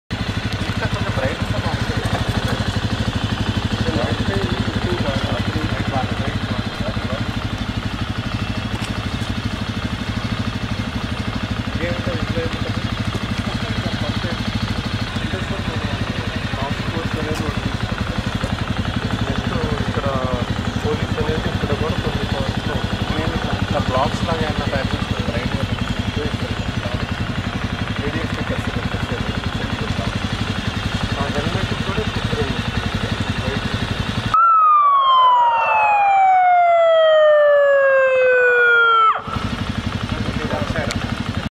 Keeping in view the increasing traffic congestion in IT hub Cyberabad a Traffic Task Force
Keeping in view the increasing traffic congestion in IT hub Cyberabad a Traffic Task Force has been formed by @cpcybd. 12 cops equipped with body-worn cameras, shoulder lights, and man pack will drive 6 custom-made motorcycles and patrol the areas.